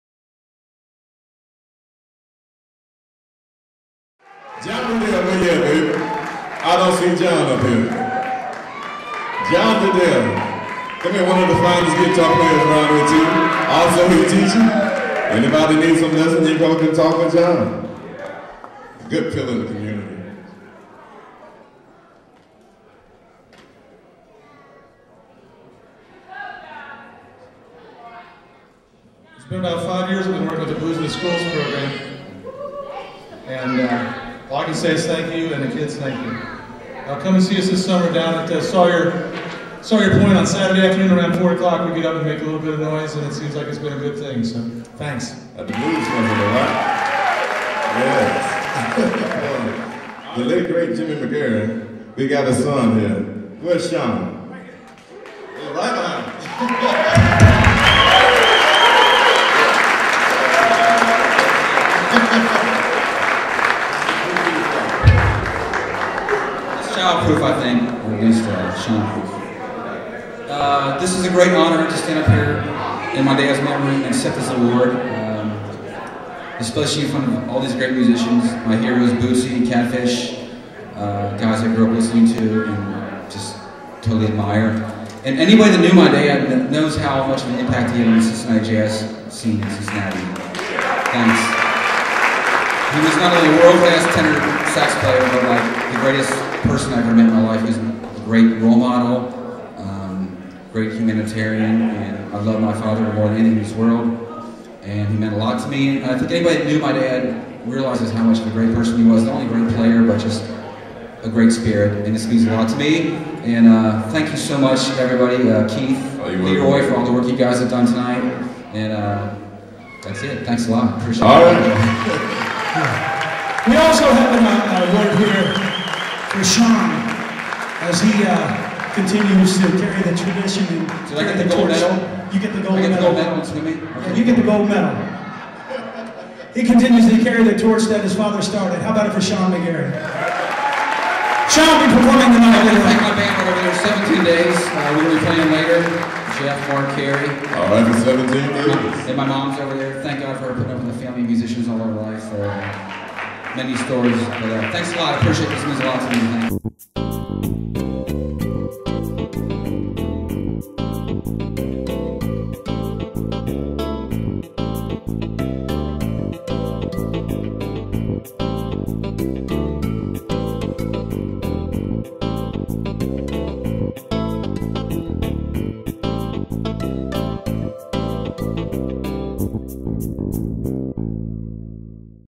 John Riddell Sean & Jimmy MaGary @ CULAN Awards 2005
Cincinnati's United Local Artists Network Inc (CULAN Inc.) CULAN's Music Masters Summit & Award Ceremony 2005